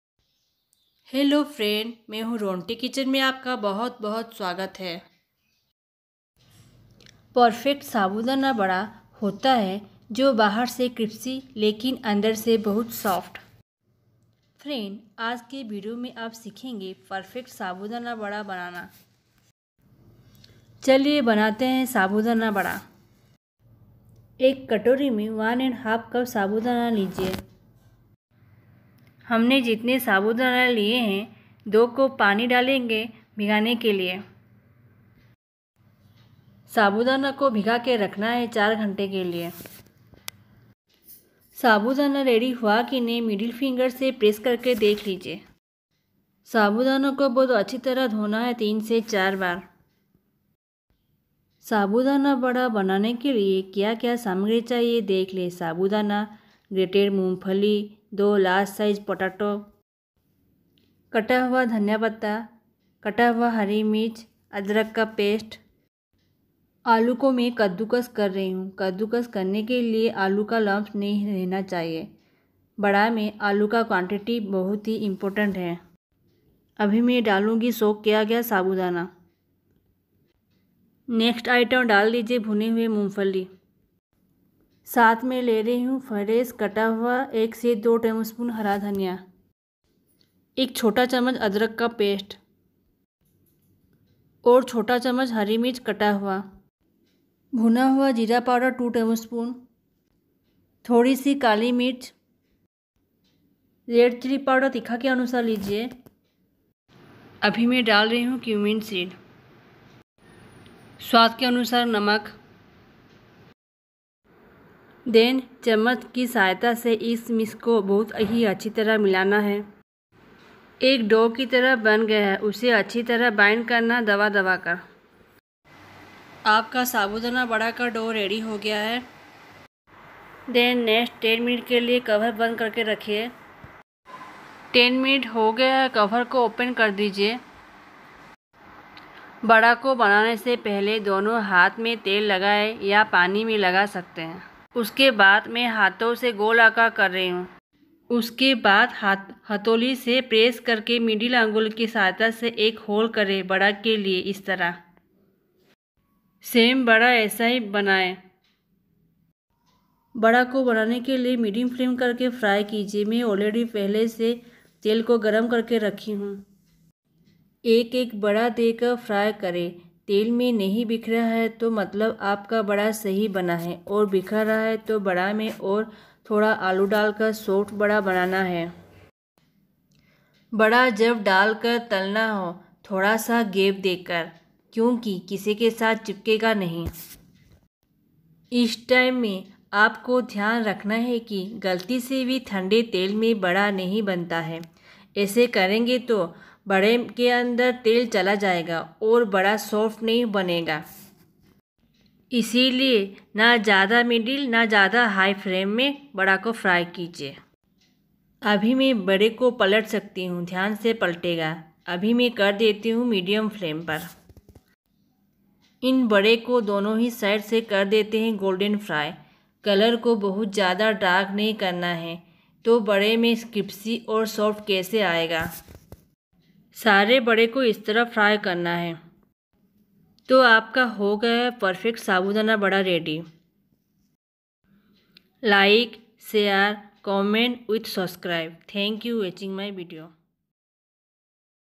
साबूदाना वड़ा रेसिपी/Sabudana vada /Sago Wada
साबूदाना वड़ा रेसिपी/Sabudana vada Recipe/Sago Wado Recipe
Sabudana vada are a popular maharashtrian snack generally prepared during fasts,soaked Sabudana or sago mixed with select spices ,crushed peanuts and boiled potatoes are shaped into vada and fried till the outside is crunchy while the inside remains soft and mushy.